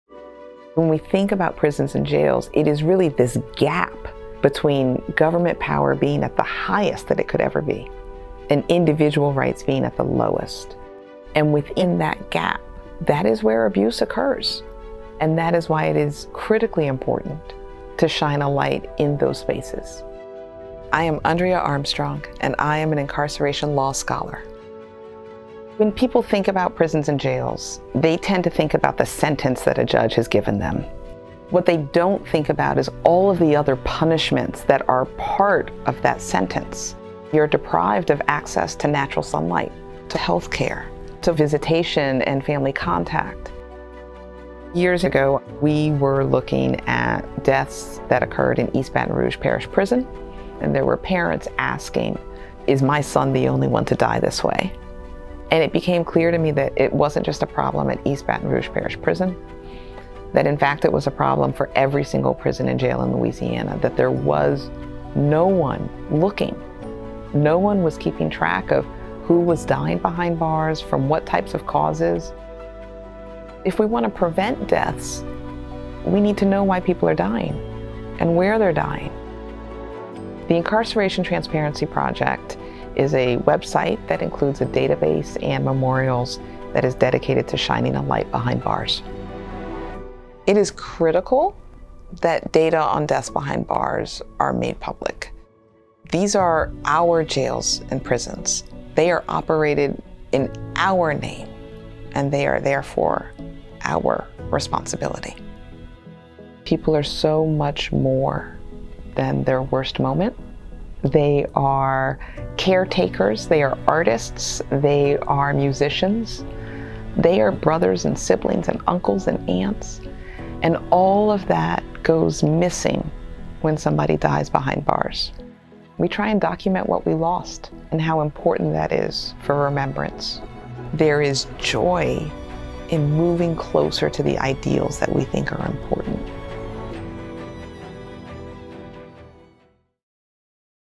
Andrea Armstrong | 2023 MacArthur Fellow
Andrea Armstrong is an incarceration law scholar and 2023 MacArthur Fellow shining a light on poor living conditions in U.S. jails and prisons.

Andrea’s Incarceration Transparency project is an online database with information and memorials about individuals who died in Louisiana prisons and jails.

Her work combines data with compelling storytelling to advocate for more humane and transparent prison conditions.

Explore her work and all the 2023 MacArthur Fellows on our website!
Video description: Andrea Armstrong, a woman with dark locs and glasses in a grey blouse, speaks about her work while in front of a bookshelf and walking through a library. Also depicted are videos from jail and prison facilities, community conversations, and images of the Incarceration Transparency website.